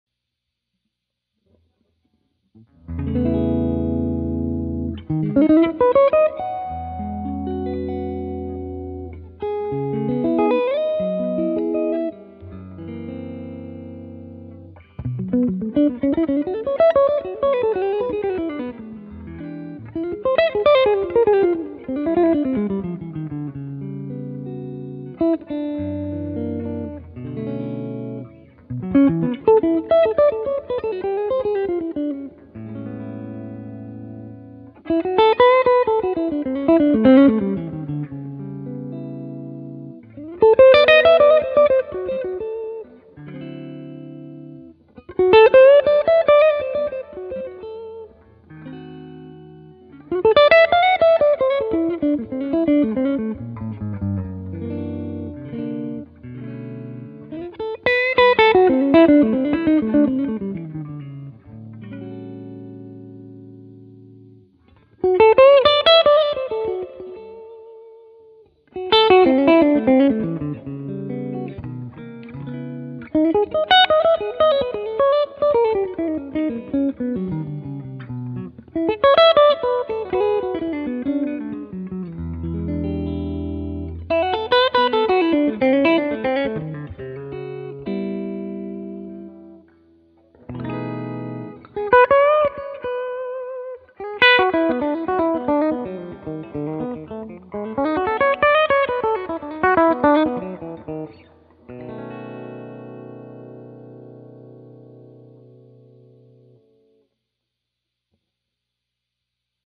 CR Guitars Features: Stephen Marchione Semi Hollow
Welcome to our CR Guitars Features series, showcasing some of the finest instruments to come through our shop! This is a Stephen Marchione Semi Hollow

Equipment Used in this Production:

- Royer R-122
- Shure SM57
- Chandler Limited TG2 Preamp